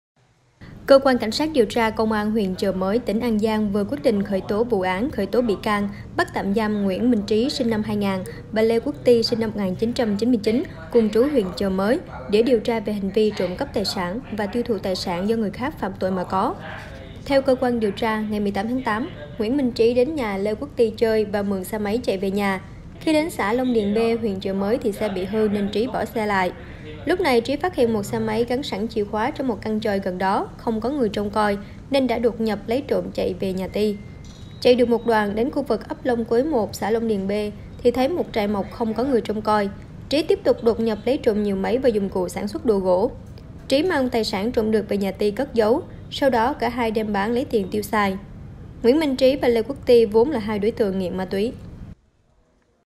Khởi tố, bắt tạm giam 2 kẻ trộm cắp, tiêu thụ tài sản | NLĐTV
Cơ quan CSĐT Công an huyện Chợ Mới, tỉnh An Giang vừa quyết định khởi tố vụ án, khởi tố bị can, bắt tạm giam Nguyễn Minh Trí (SN 2000) và Lê Quốc Ty (SN 1999), cùng trú huyện Chợ Mới, để điều tra về hành vi trộm cắp tài sản và tiêu thụ tài sản do người khác phạm tội mà có

* Thực hiện: Nghiêm Túc - Hồng Hải - Thanh Long